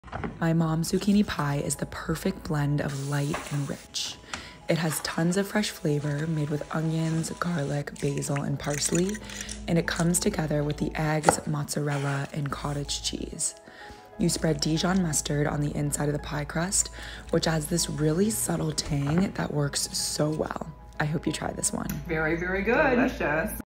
Mom’s zucchini pie ✨
For the Sautéed Zucchini:▪️2 Tbsp Butter▪️1 Cup Onion, diced small▪️2 Cloves Garlic, minced▪️4 Cups Zucchini, sliced in thin rounds▪️1/2 tsp Pepper▪️1/2 tsp Salt▪️1/2 tsp Dried Oregano▪️
For the Pie:▪️2 Eggs▪️1 Cup Mozzarella▪️1 Cup 4% Cottage Cheese▪️2 Tbsp Fresh Basil, chopped▪️3 Tbsp Fresh Parsley, chopped▪️2 tsp Dijon Mustard▪️1 Pie Crust▪️
For Serving:▪️Fresh Parsley, minced▪️Parmesan Cheese, grated▪️
🌱Vegan Modification: Sub in 2 cups vegan mozzarella for the mozzarella and cottage cheese, sub in olive oil for butter, and leave out the Parmesan
1️⃣ Preheat oven to 375°F
2️⃣ In a pot over medium high heat, sauté onions and garlic on butter until onions are translucent (2-3 min); add the zucchini, salt, pepper, and oregano and sauté stirring often for 6-8 minutes until zucchini is tender (but not completely falling apart) and most of the liquid has cooked off (if there is a lot of excess liquid, drain it off); turn off the heat
3️⃣ In a separate large bowl, beat the eggs and mix with the mozzarella, cottage cheese, basil, and parsley; add the zucchini mixture and stir to combine
4️⃣ Spread the pie crust into a pie pan and brush Dijon mustard in the inside of the pie shell; pour in the filling and arrange zucchini rounds on the top so it looks pretty (optional)
5️⃣ Bake 35-40 mins until a knife comes out clean from the center (if the exposed edge of the pie crust looks like it's over browning you can cover it with foil, but we didn't have to)
6️⃣ Let cool for 5 mins to set and then sprinkle with parsley and Parmesan and enjoy!
⏲25 minute prep time + 40 minute cook time
Recipe makes 1 pie (serves 4-6 people)